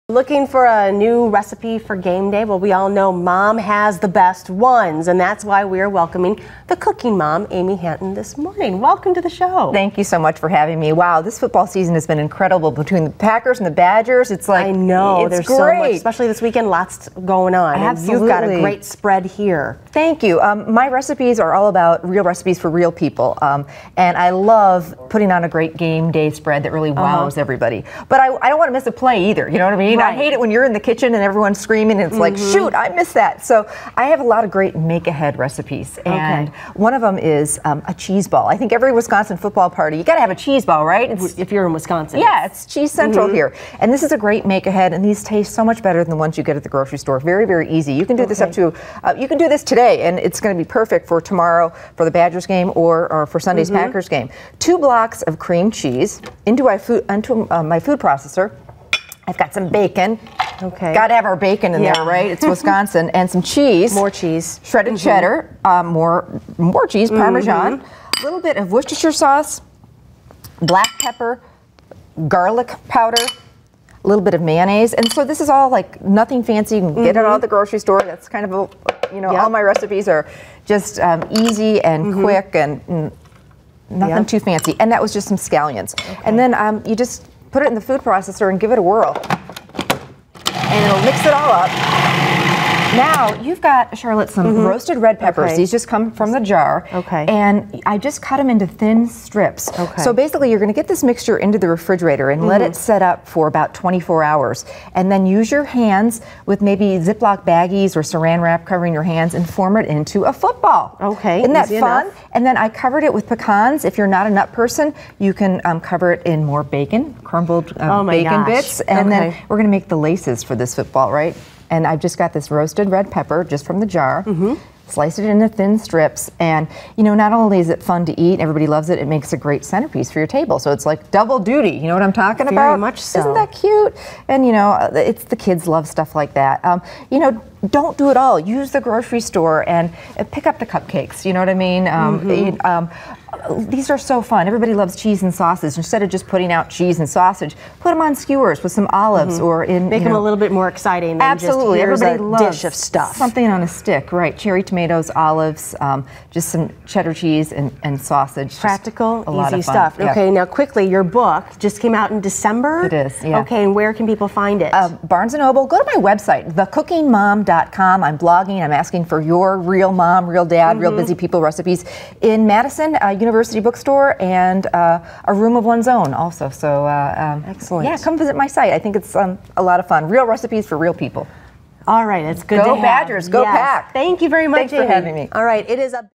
'Cooking Mom' Gives Some Game Day Recipes
"Cooking Mom" Amy Hanten offers some game day recipes